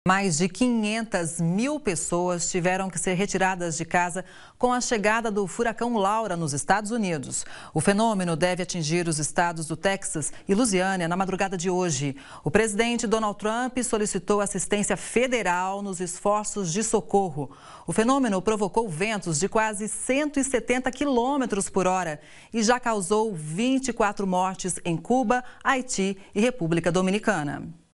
Estados Unidos se preparam para chegada do Furacão Laura
O furacão Laura deve ganhar força rapidamente e chegar à categoria 4 nesta quarta-feira (26), seguindo uma rota direta para atingir o Texas e a Louisiana, na Costa do Golfo dos Estados Unidos.